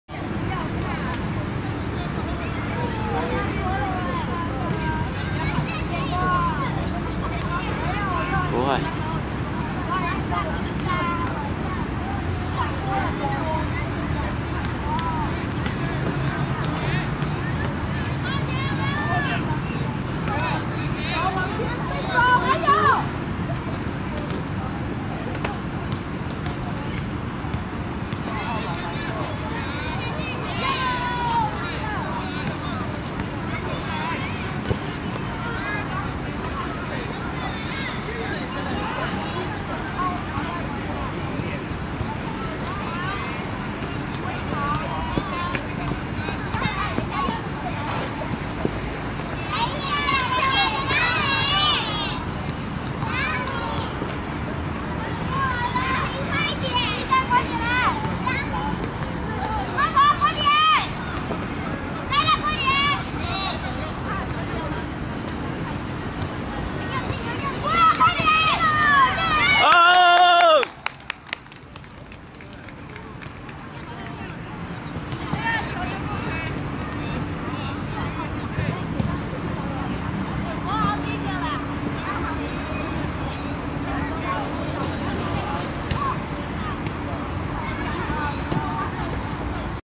U10學童杯中區預賽-黎明vs華盛頓 精彩片段一
黎明國小女足隊2017第四屆